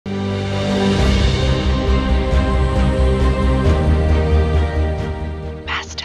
Mazda CX-5 2025, exclusiva versión Carbon edition
El carácter deportivo de sus detalles en color negro se conjuga de manera armónica con la apariencia elegante de sus líneas exteriores, convirtiendo a esta SUV en una auténtica pieza de exclusividad y sofisticación.